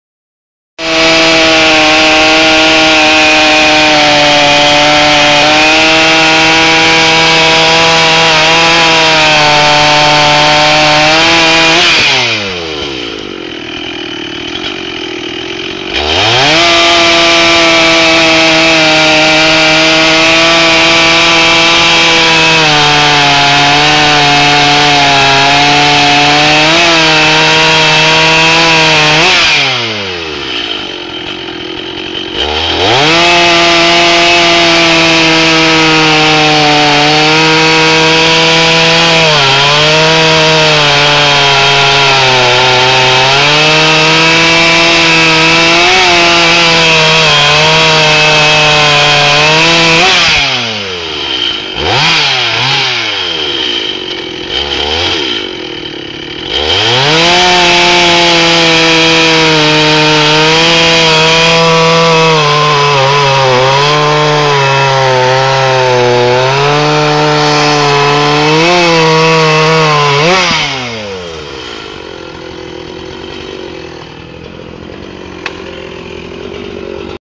Dolmar 7900 HD Filter in Oak
This is a large red oak that split in half from a recent storm. I running my Dolmar 7900 with the HD filter upgrade, 24" bar.